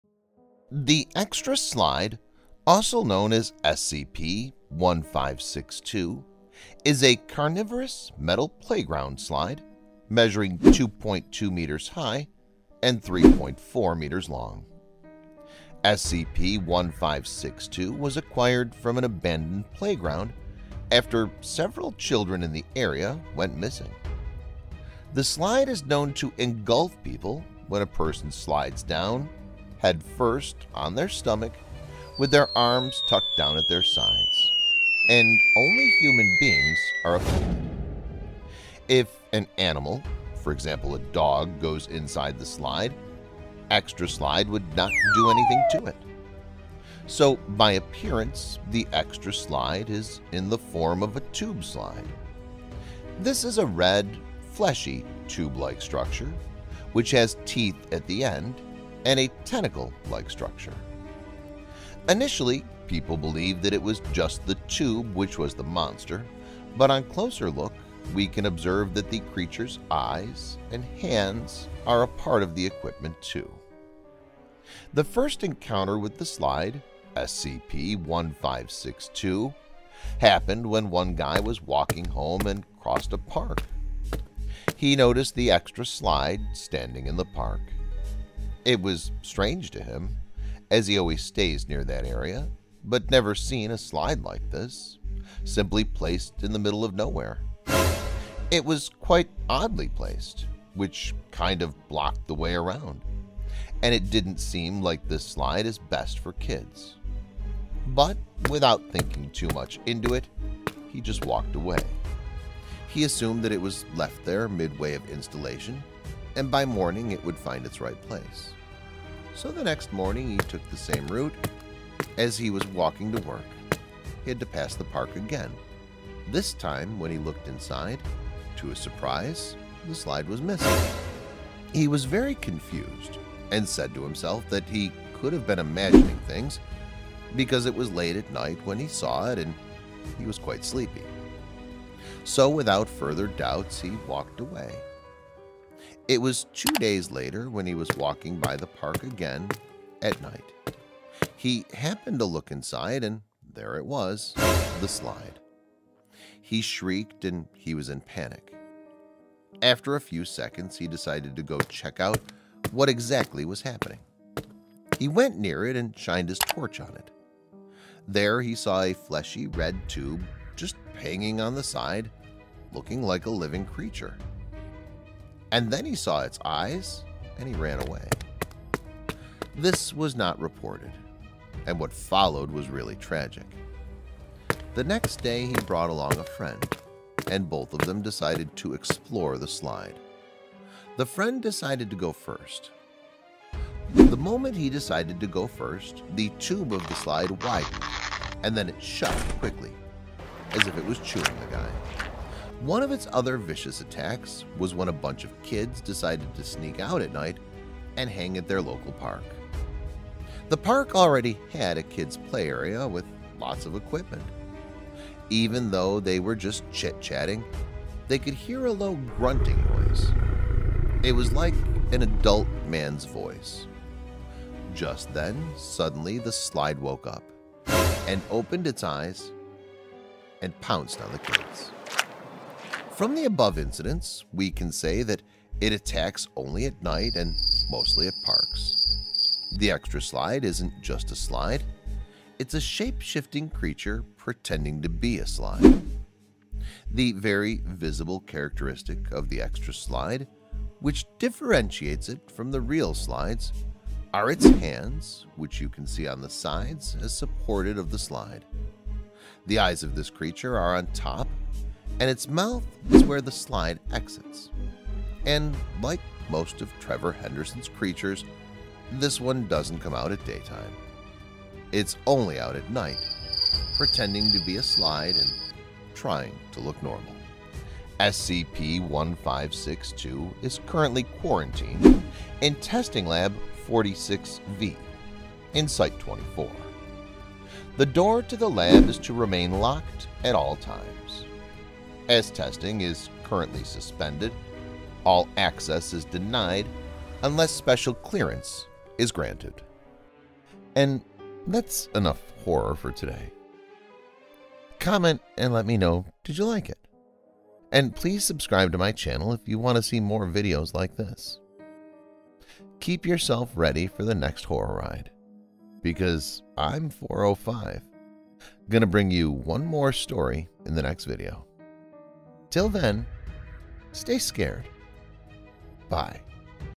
SCP 1562 | The Carnivores Slide : SCP Animation
SCP 1562 | The Carnivores Slide : SCP Animation is about SCP 1562 aka “The Extra Slide” is a man eating playground slide. SCP 1562 is a red tube like structure. SCP 1562 or The Carnivores Slide is named as Extra Slide because it acts like an additional slide. Behind that camouflage, SCP 1562 is a man eating monster in the SCP world. SCP Foundation personnels are still researching on The Extra Slide to know more about it.